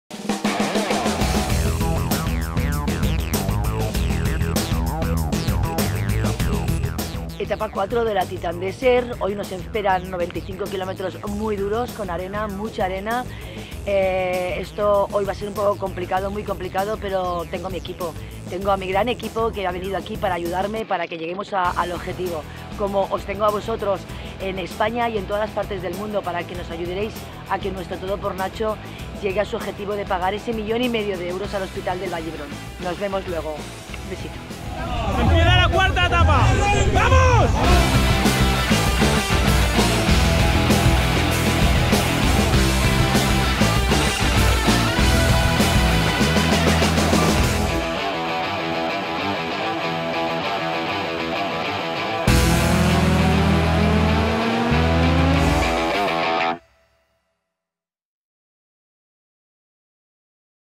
4ª Etapa.- Arena y más arena
100% Desierto bajo nuestros pies, arenales, dunas, tramos a pie, pero ante todo con la mirada puesta en el objetivo de hoy: llegar a meta con el TodoPorNacho por bandera.
No nos va a parar nada ni nadie, y lo vais a ver.

¡¡Vamos!!